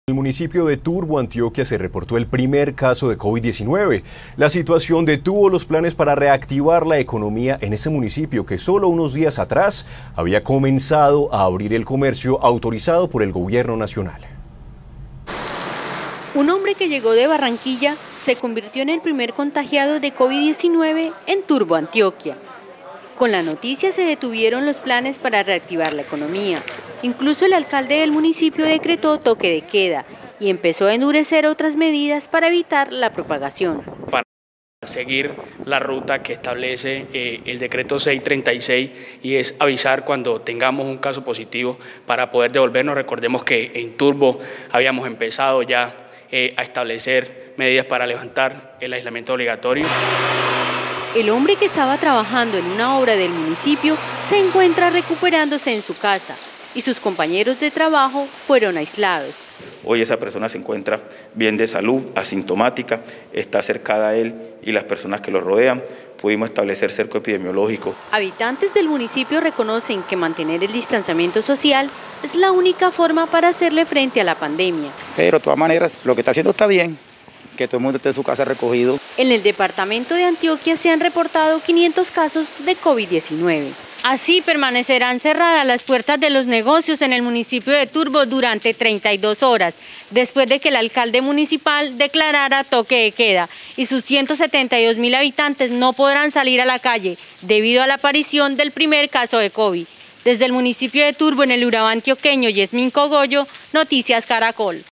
Primer caso de coronavirus en Turbo: decretan toque de queda y frenan la reapertura del comercio
Por un hombre que llegó de Barranquilla y que dio positivo para COVID-19 se detuvieron los planes para reactivar la economía. Endurecen medidas.